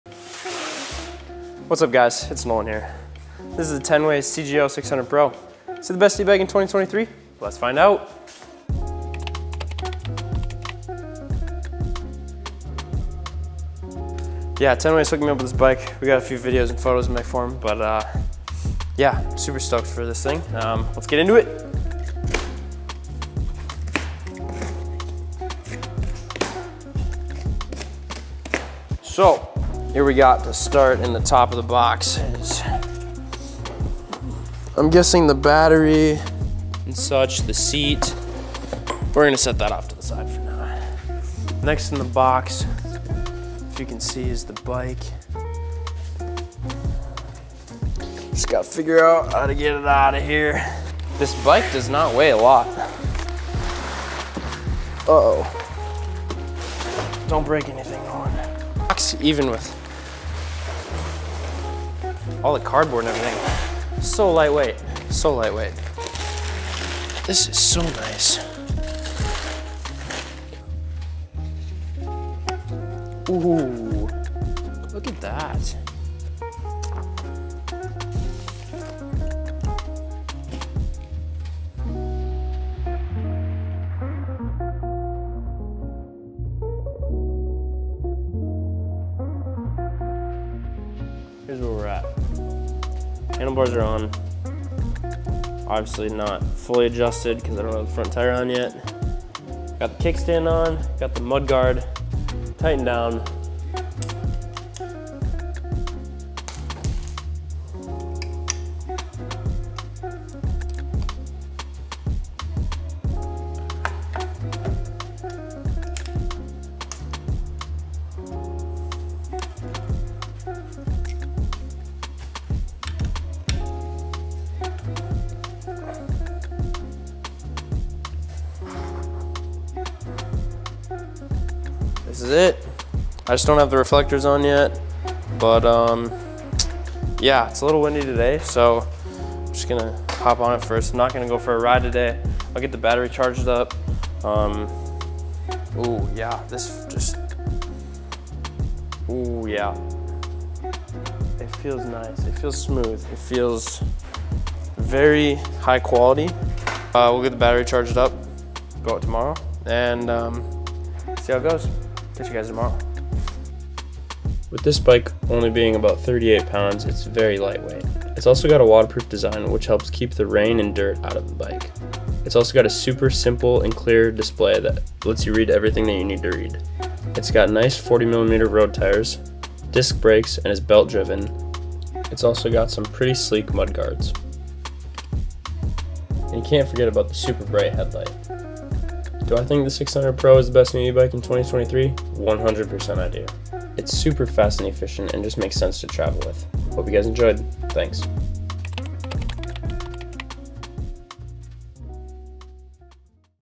BEST E-BIKE IN 2023? | Tenways CGO600 Pro
Is this the best e-bike in 2023? I think so! The Tenways CGO600 Pro is super fast, efficient, quiet, comfortable, lightweight, durable, and sleek. All of these set it apart from other e-bikes out there.

Here is a review and unboxing video of the Tenways CGO600 Pro! It has been a blast riding and testing out this bike while creating content! I highly recommend getting one to anyone out there looking for a super high quality e-bike to travel with!

Current Gear:

Sony A7IV
Sony Zeiss 16-35mm f/4
Sony Zeiss 85mm f/1.8